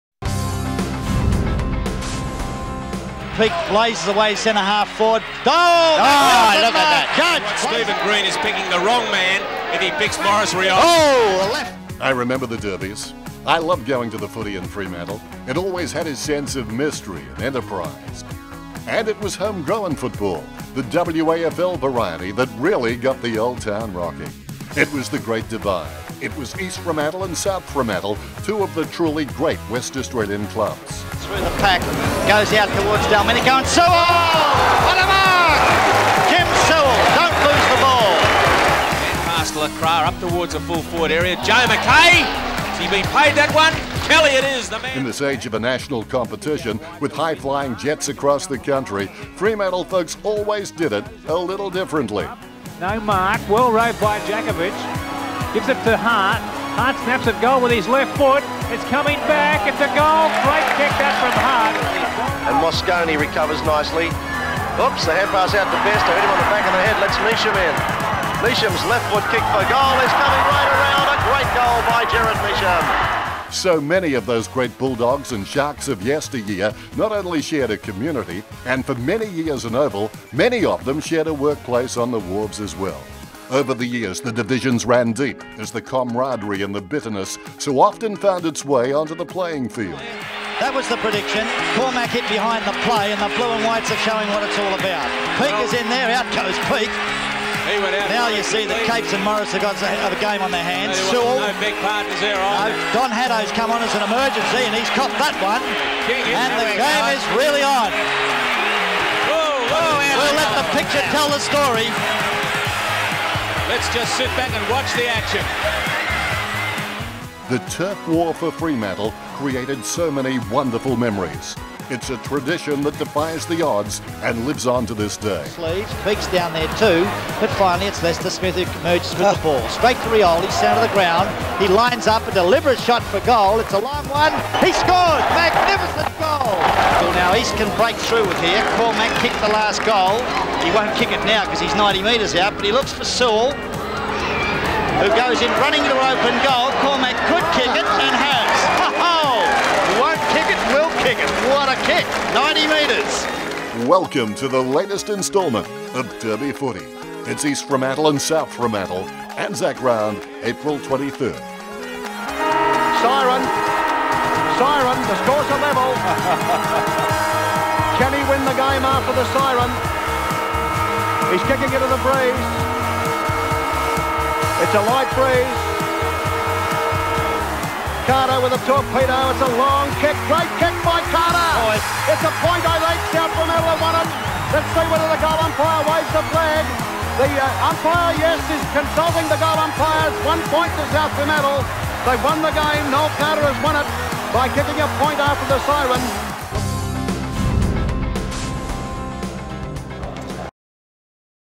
WAFL Fremantle Derby Heritage with Dennis Cometti
Dennis Cometti opens our eyes to a bit of WAFL heritage and the famous 1979 Fremantle Derby Grand Final.